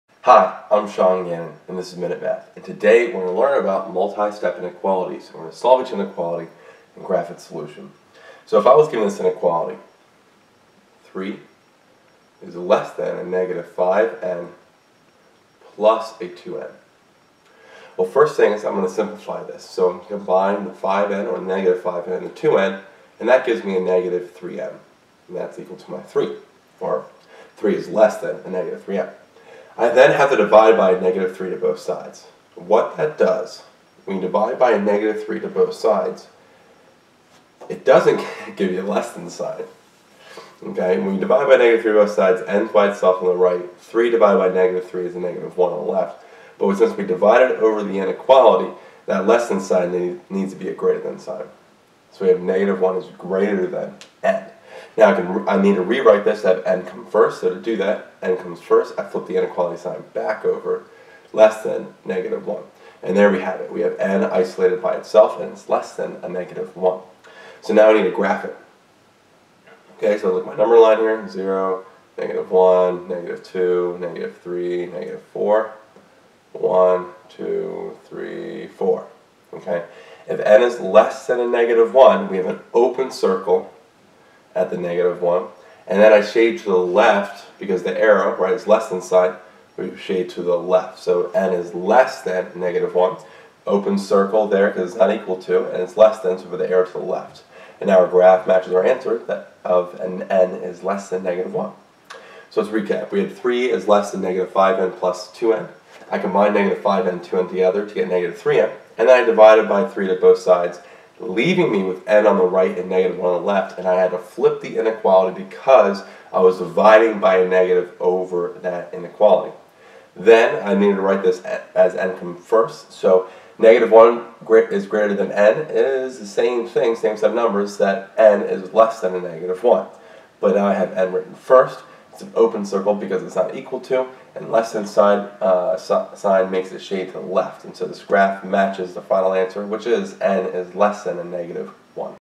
Solve and graph 3‹-5n+2n
In this math video lesson on Multi-Step Inequalities, I solve 3 less than -5n+2n and graph its solution.